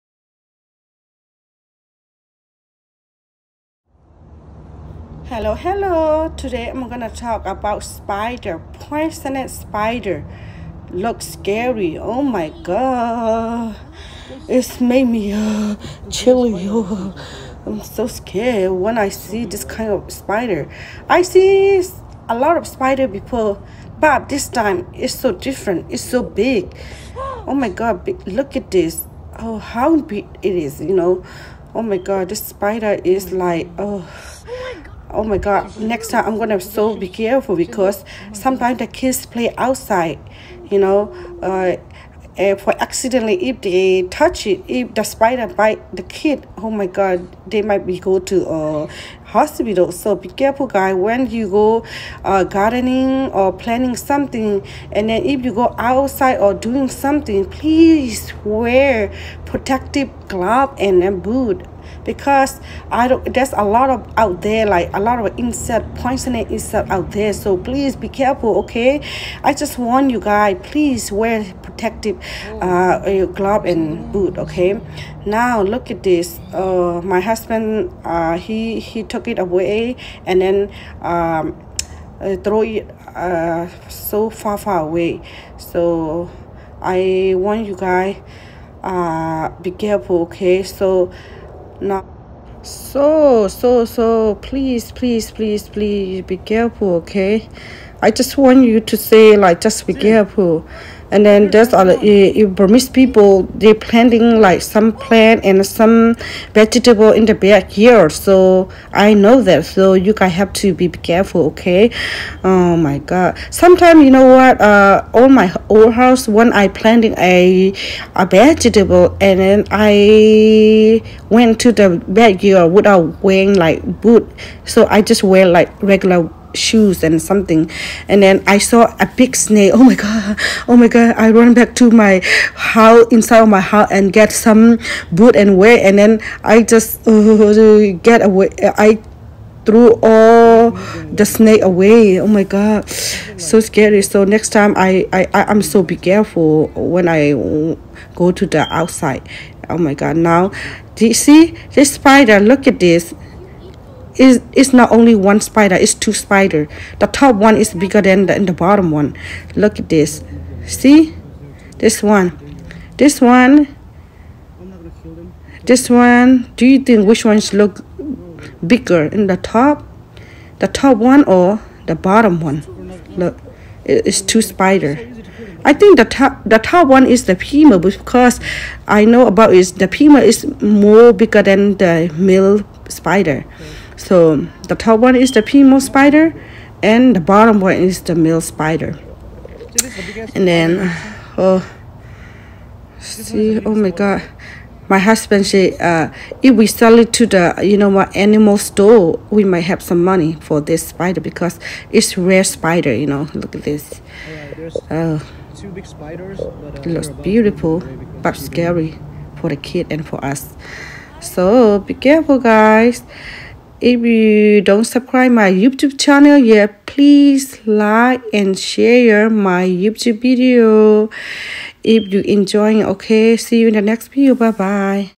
Venom couple spiders
Omg I Found a beautiful couple spiders in the backyard
Mother’s in law ..
 Please be careful out there if u are going to do something outside backyard
Wear protective gloves and boots!!